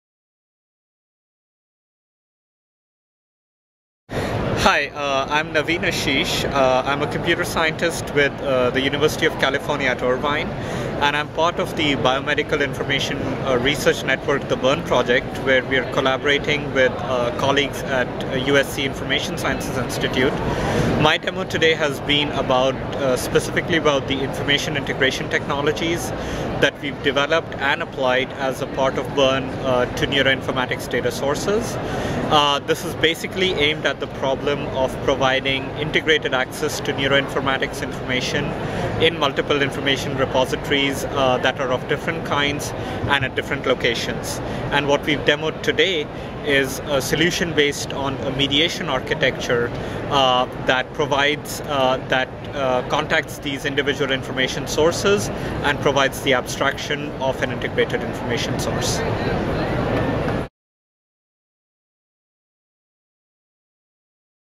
INCF at SfN 2010: Naveen Ashish introduces his BIRN demo
Each year at SfN Neuroscience, INCF hosts neuroinformatics demos in the INCF booth. One of this year's demos showed data federation in BIRN; here Naveen Ashish introduces the demo.